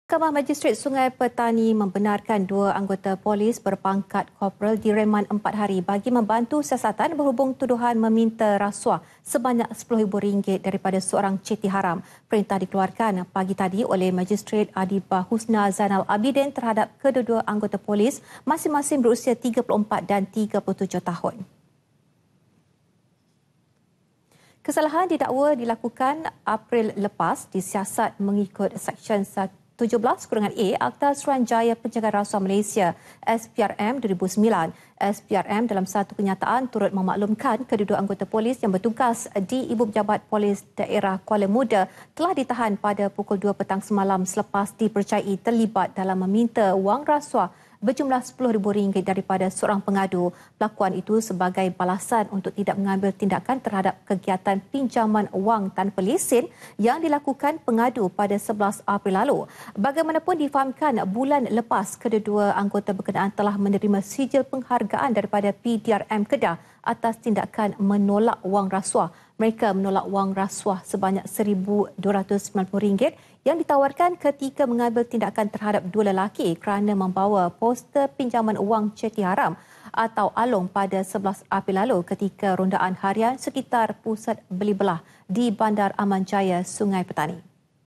RASUAH – 2 KOPERAL DIREMAN EMPAT HARI [5 JULAI 2017]
Mahkamah Majistret Sungai Petani, MEMBENARKAN dua anggota polis berpangkat koperal direman EMPAT HARI bagi membantu siasatan berhubung tuduhan meminta rasuah sebanyak 10 ribu ringgit daripada seorang ceti haram.

Perintah dikeluarkan, pagi tadi oleh Majistret Adibah Husna Zainal Abidin terhadap kedua-dua anggota polis masing-masing berusia 34 dan 37 tahun.